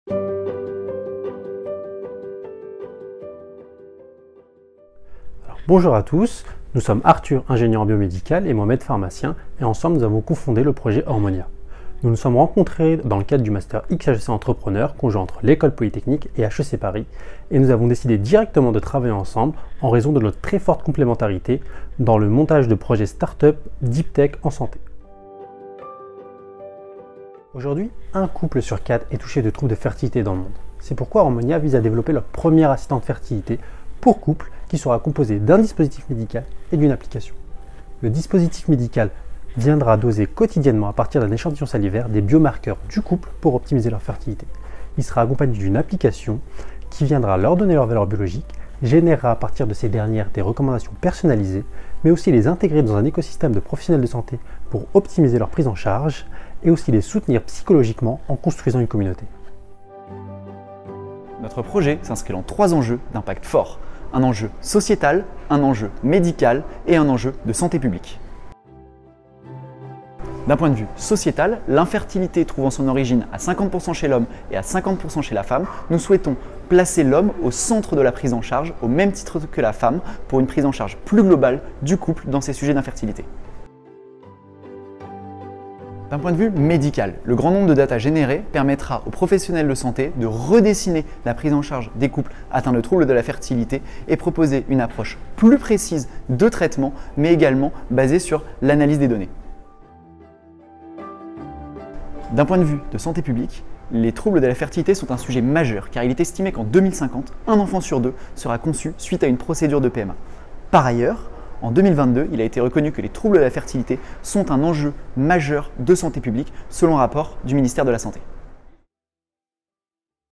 HORMONE.IA (75)
HORMONE.IA développe le premier assistant de fertilité pour couple. Ce dispositif médical (DM) permettra d'accompagner hommes et femmes dans leur parcours de fertilité.

- Projet porté par Arthur Sérès